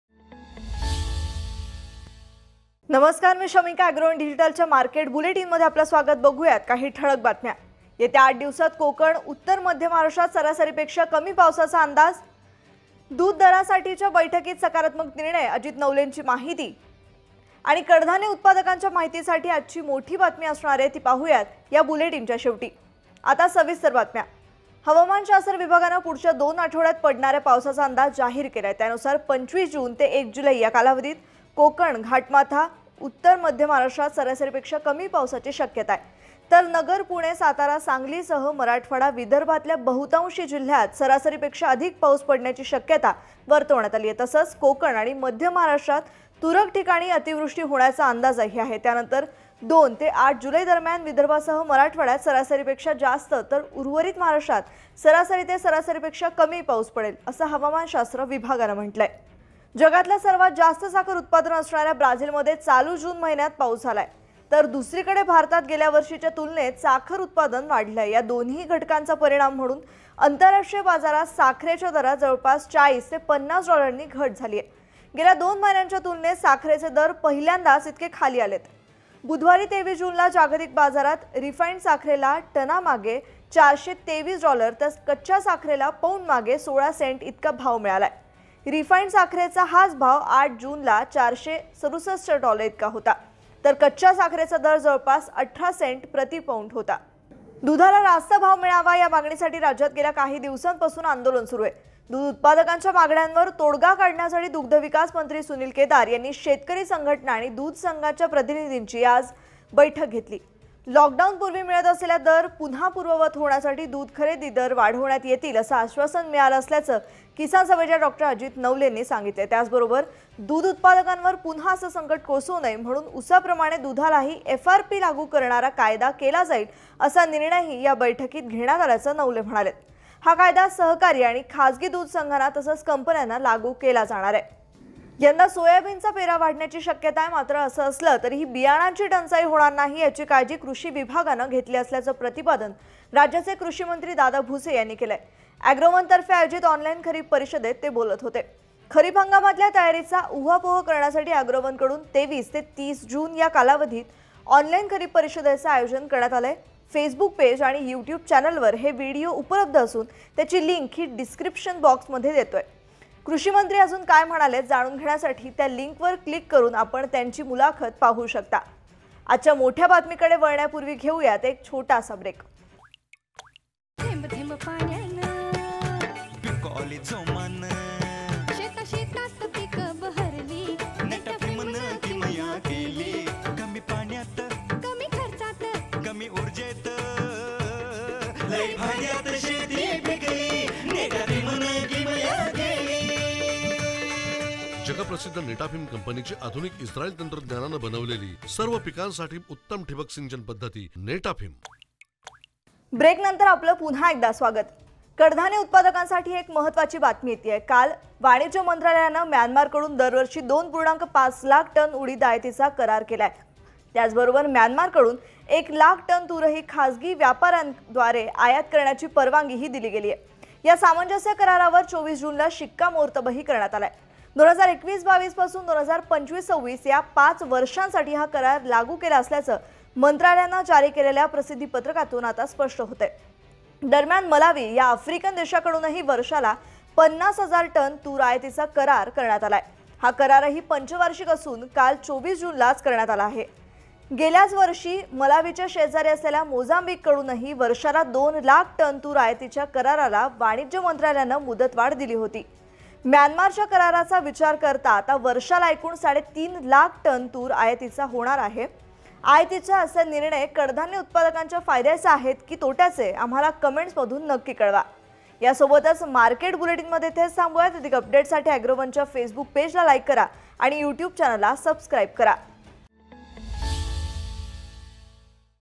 Myanmar मधून Pulses Import साठी करार | Pulses Import in India || ॲग्रोवन
कडधान्य उत्पादकांच्या माहितीसाठी आजची मोठी बातमी आहे. ती नेमकी काय, जाणून घेऊयात आजच्या मार्केट बुलेटिनमधून.
आमच्याशी जोडून राहण्यासाठी भेट द्या